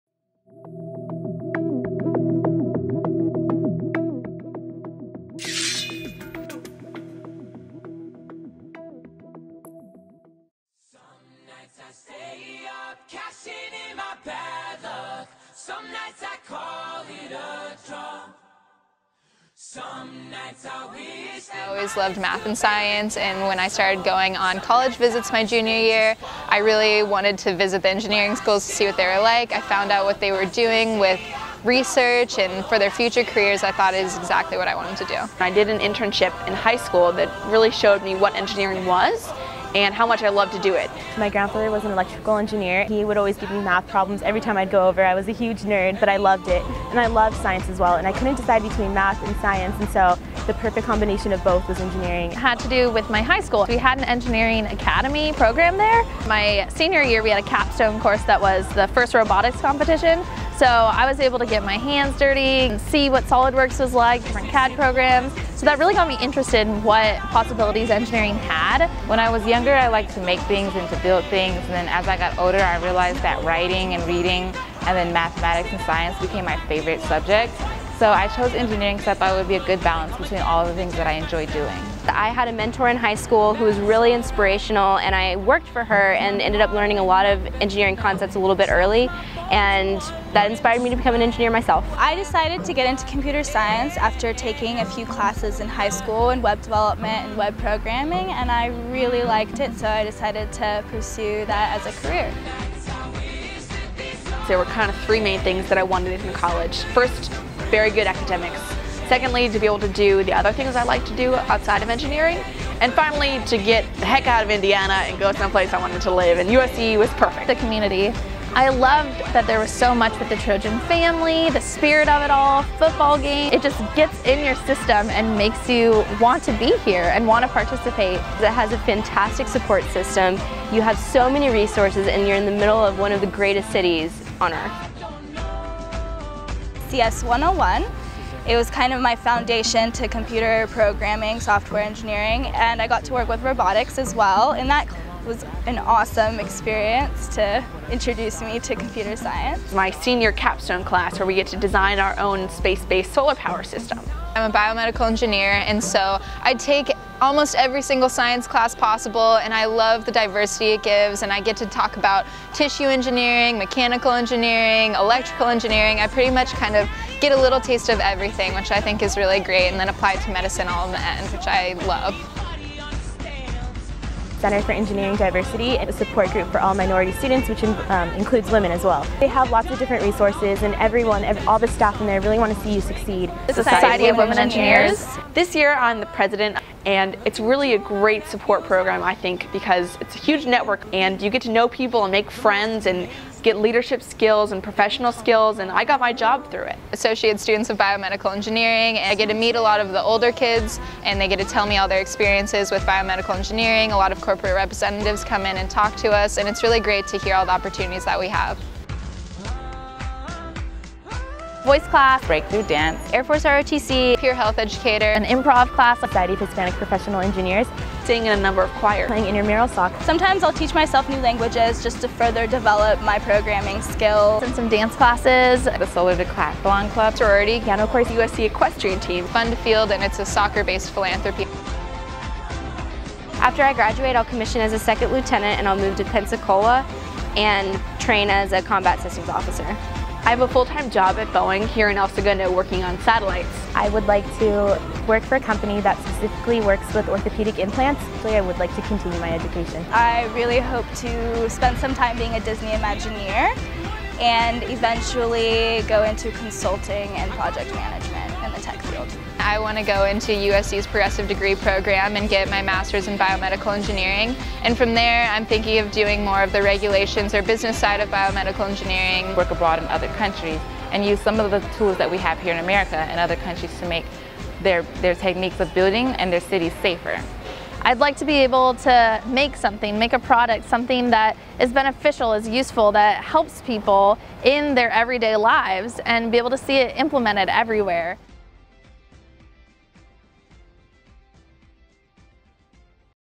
Women in Engineering at USC
We asked some of our current undergraduate female students at Viterbi to share with us why they chose engineering, their experiences as women in engineering at USC, and their goals for the future.